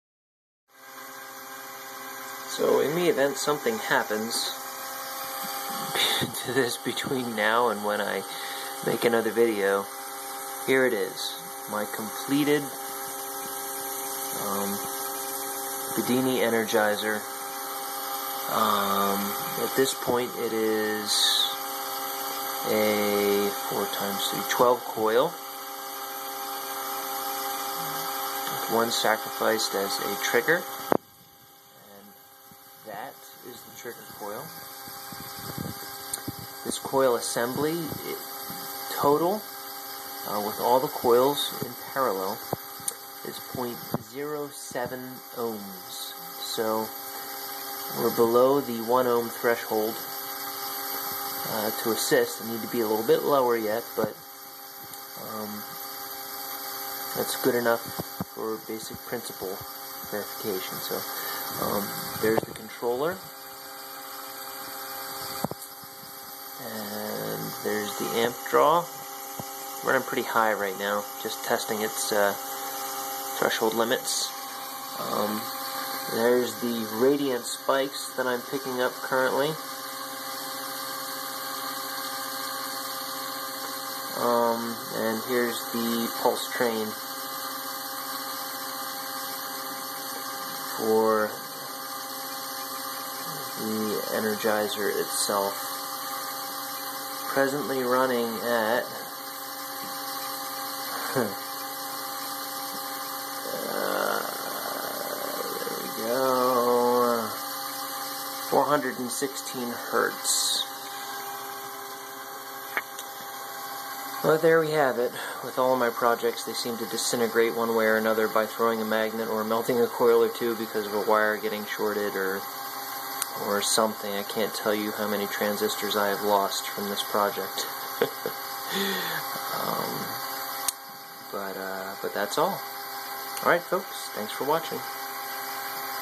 Bedini 12 coil
New bedini multicoil energizer. 12 coils +1 trigger coil. 3 magnet rotor all north facing out placed at 120 degree spacing around a 1 inch diameter.